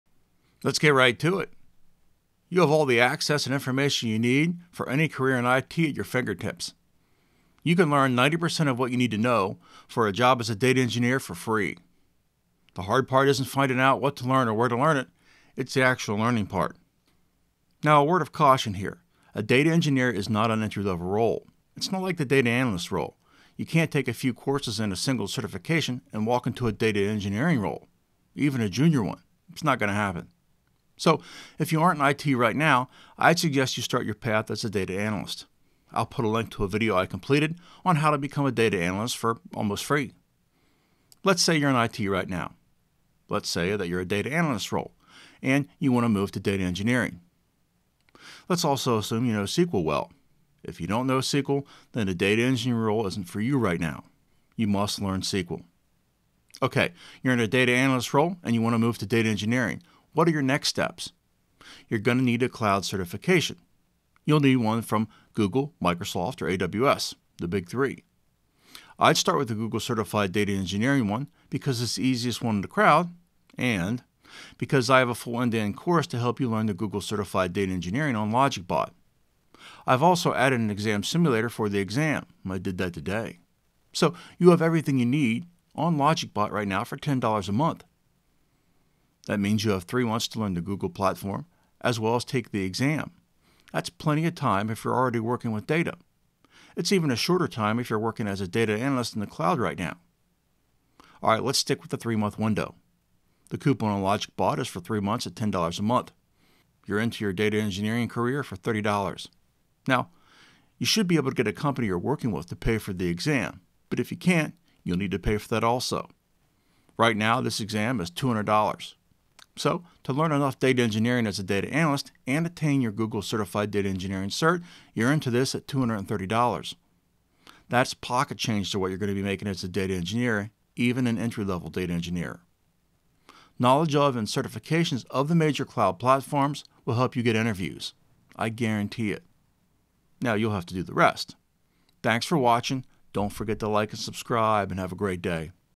How to Become a Data Engineer for FREE (Mostly Free)
Let's learn how to become a data engineer for mostly free.